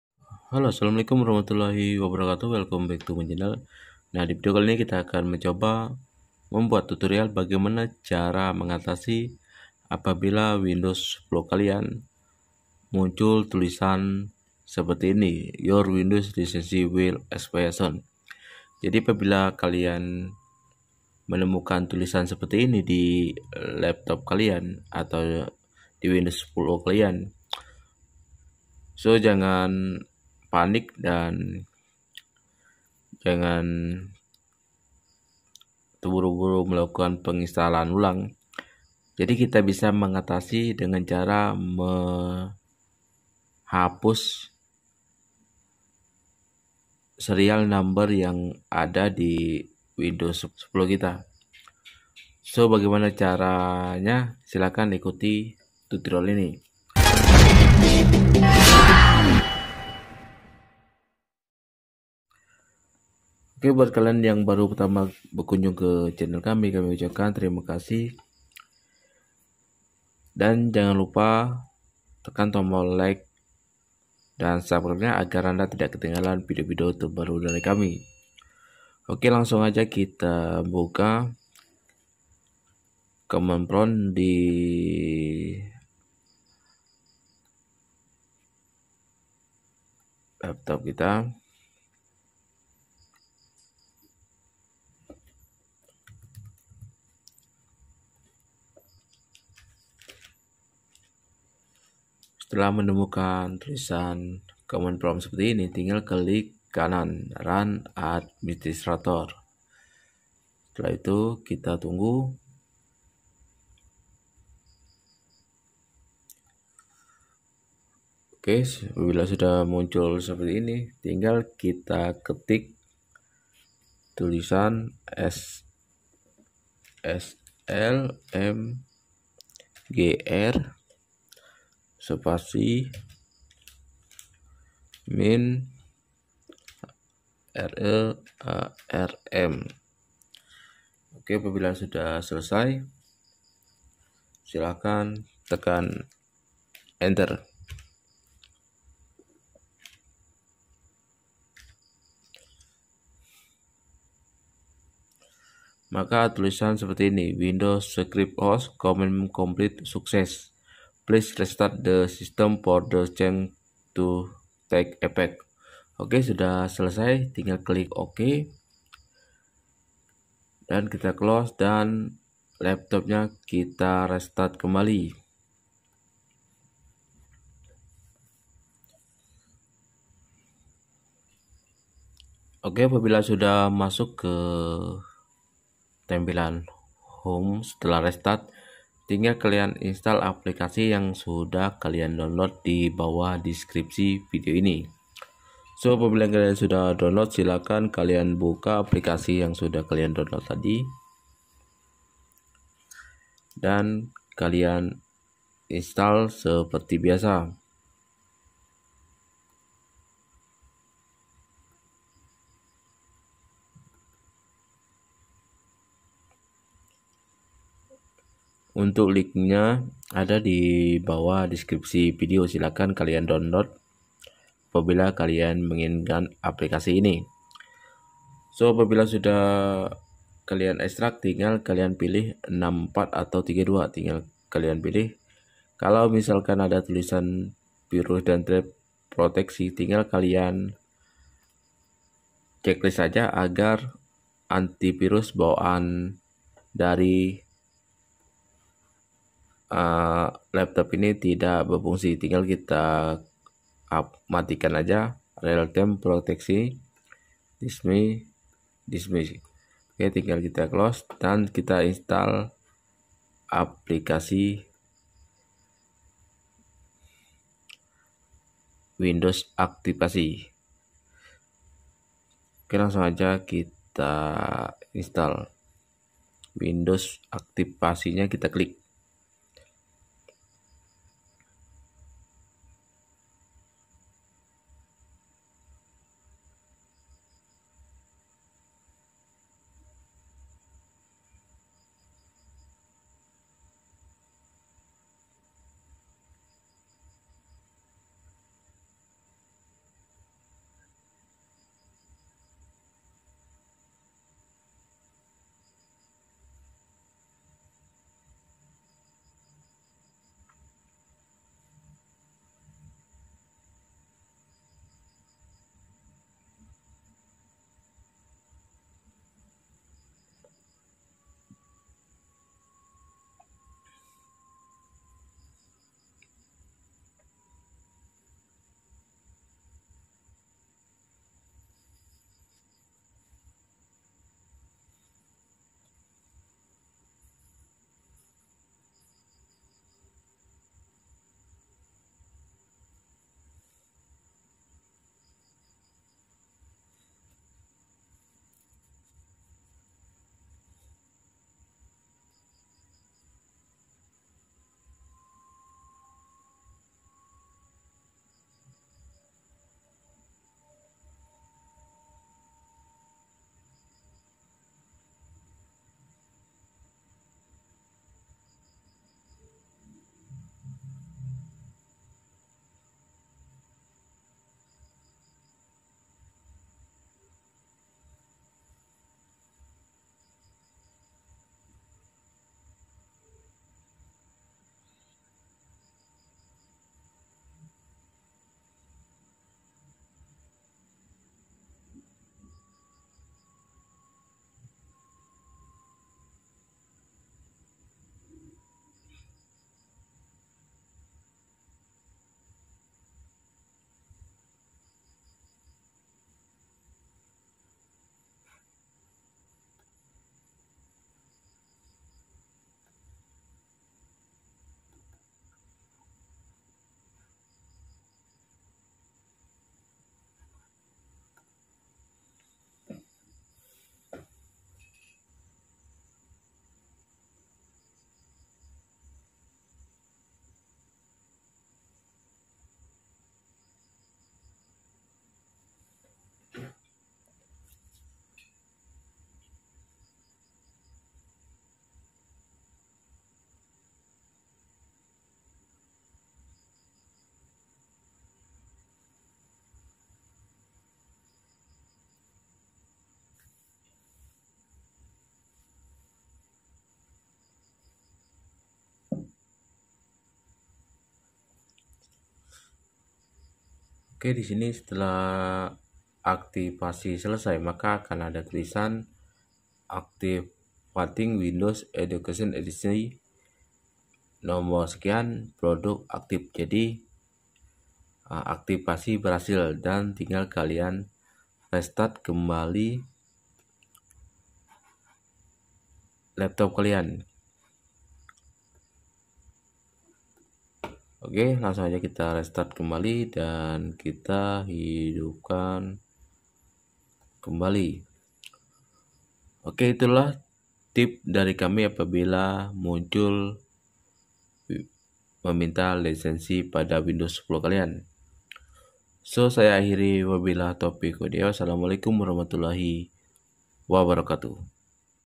Your Windows License Will Expire Soon / cara mengatasi Your Windows License Will Expire Soon
divideo kali ini, tutorial kita akan mencoba membuat tutorial bagaimana cara mengatasi windows 10 kalian ada bacaan your windows license will expre soon

JANGAN LUPA RATE, COMMENT, SUBSCIBE & SHARE